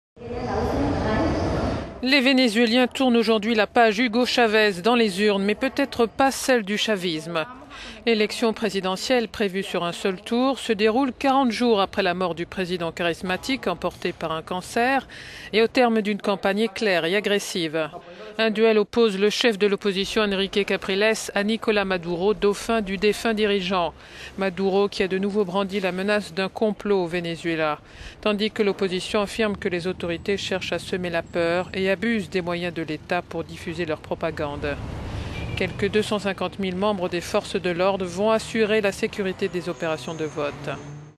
Election présidentielle: jour J au Venezuela
Les Vénézuéliens tournent aujourd'hui la page Hugo Chavez dans les urnes, mais peut-être pas celle du chavisme.

L'élection présidentielle, prévue sur un seul tour, se déroule quarante jours après la mort du président charismatique emporté par un cancer, et au terme d'une campagne éclair et agressive.

Un duel oppose le chef de l'opposition Henrique Capriles à Nicolas Maduro, dauphin du défunt dirigeant.  Maduro qui a de nouveau brandi la menace d'un complot au Venezuela tandis que l'opposition affirme que les autorités cherchent à semer la peur et abusent des moyens de l'Etat pour diffuser leur propagande.

Quelques 250 000 membres des forces de l'ordre vont assurer la sécurité des opérations de vote.

Avec AFP et Reuters